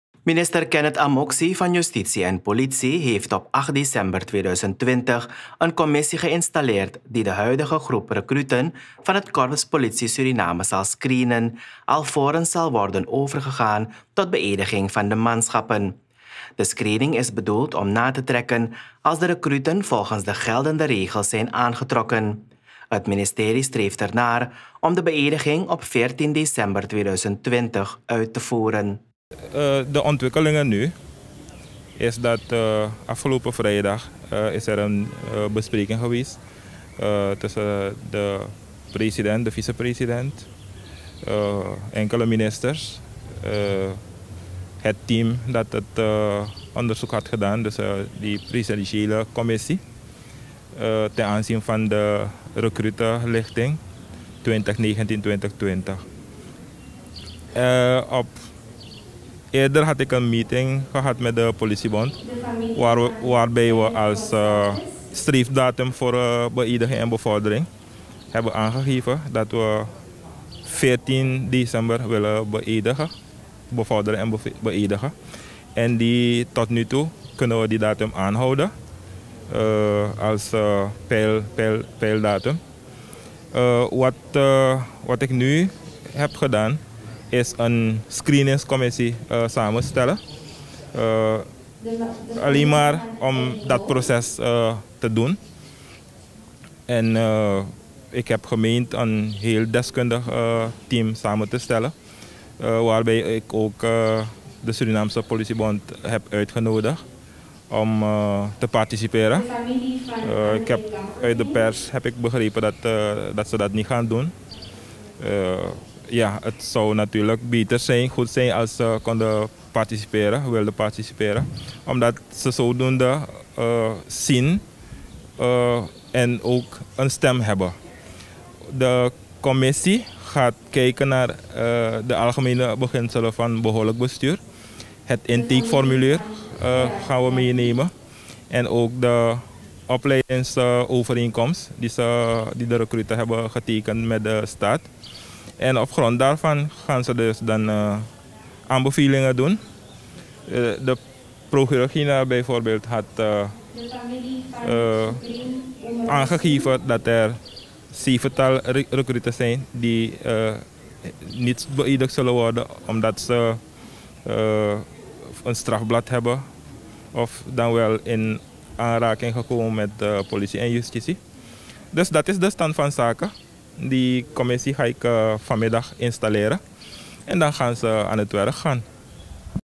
MINISTER AMOKSI INSTALLEERT COMMISSIE VOOR SCREENING REKRUTEN
Minister Kenneth Amoksi van Justitie en Politie heeft op 8 december 2020 een commissie geïnstalleerd die de huidige groep rekruten van het Korps Politie Suriname zal screening, alvorens zal worden overgegaan tot beëdiging van de manschappen. De screening is bedoeld om na te trekken als de rekruten volgens de geldende regels zijn aangetrokken. Na rapportage van de commissie zal het ministerie overgaan tot de beëdiging van de rekruten die op correcte wijze zijn toegelaten tot de opleiding. Het ministerie streeft ernaar om de beëdiging op 14 december 2020 uit te voeren.